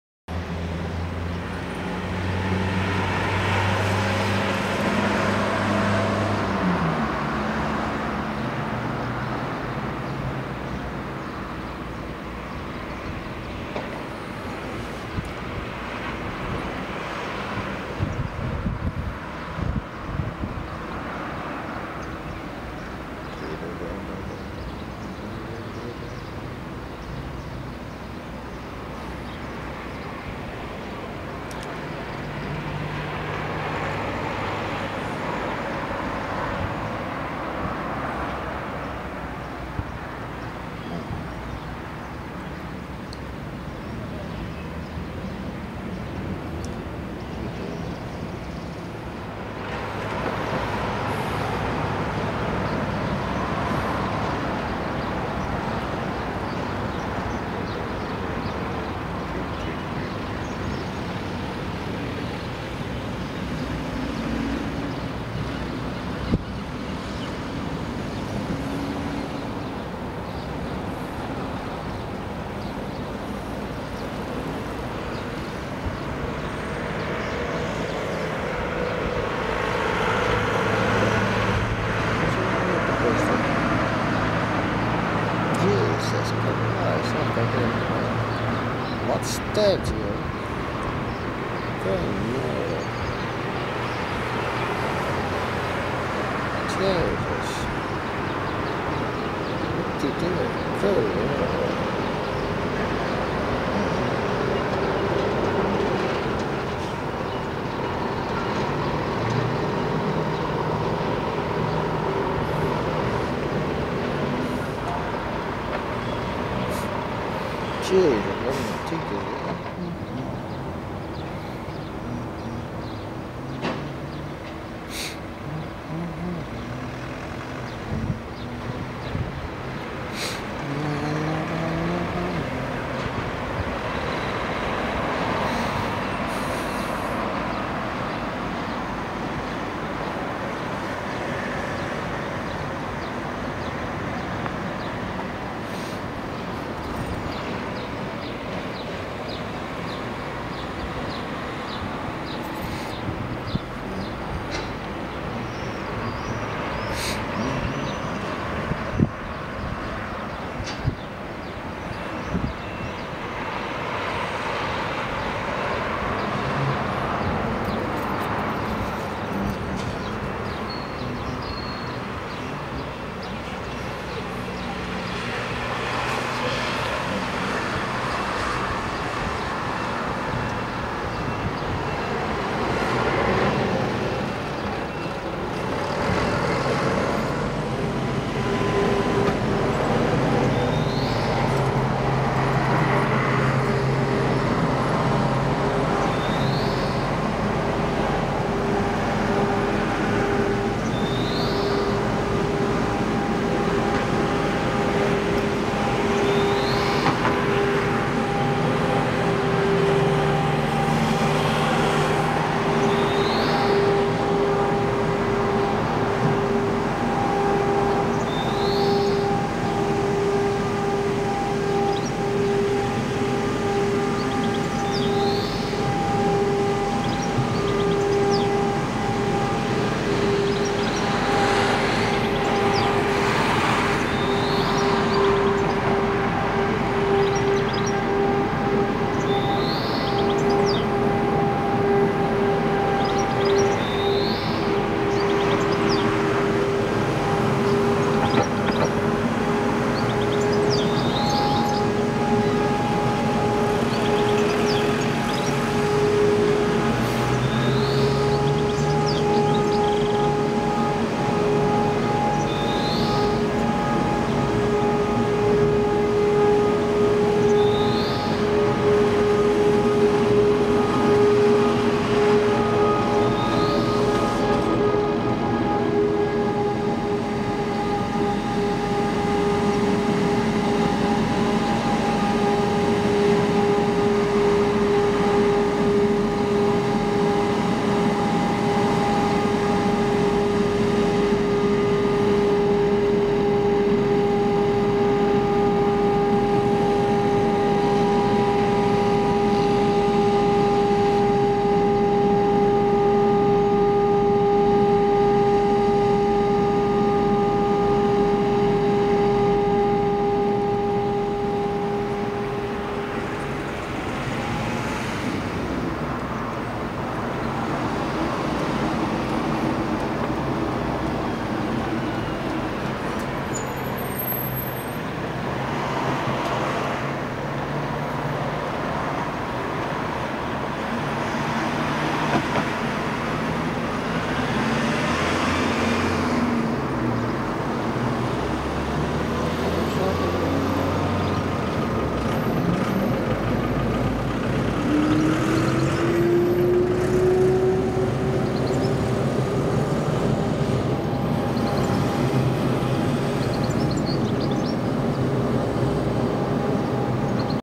White Stork - Ooievaar
Portugal - Algarve - Portimao - 2011 - White Stork - Ooievaar on the nest with Juvenils -
Juveniels